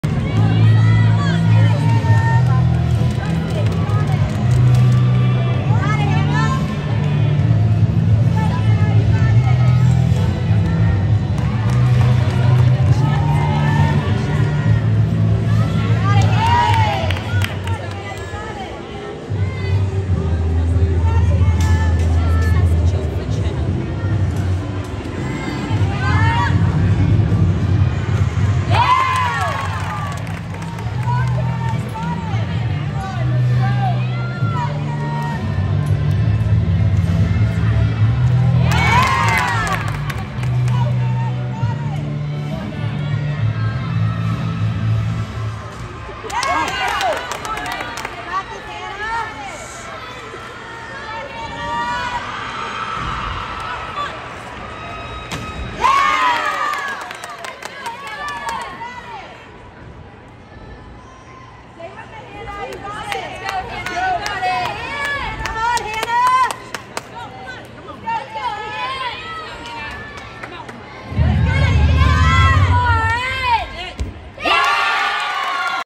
Hannah Scheible, AA Flames Beam 2023 DEV Nationals
score: 9.95 National Champion